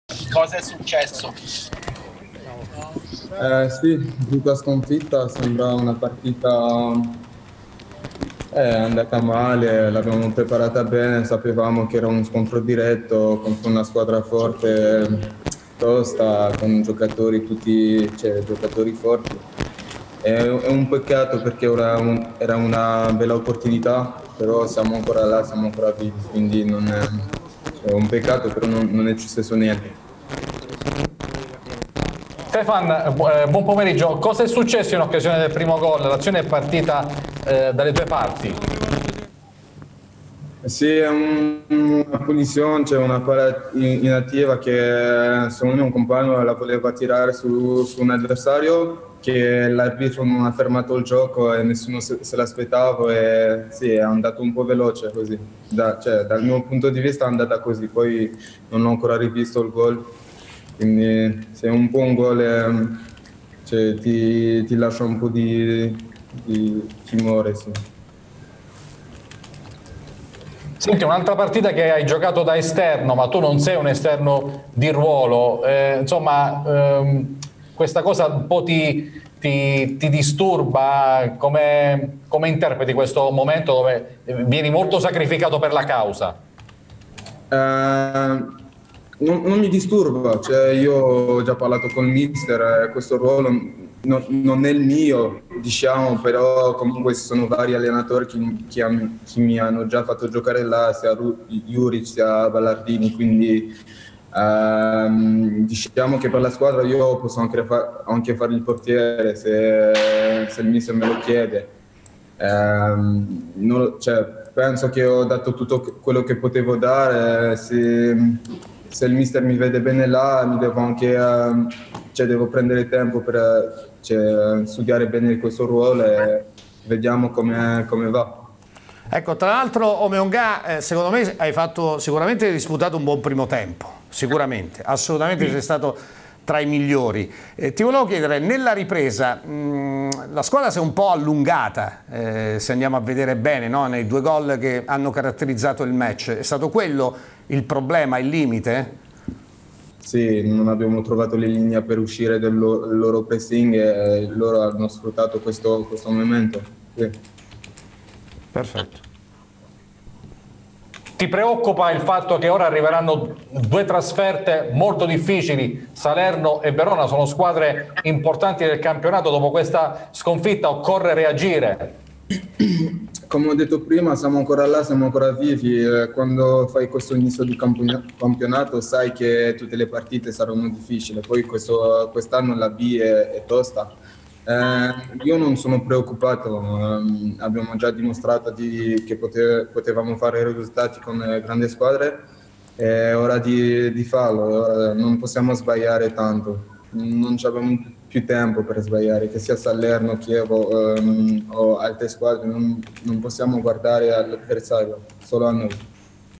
Pescara - Cremonese 0-2: Stephane Omeonga
Conferenza post gara del centrocampista del Pescara, anche oggi impiegato da esterno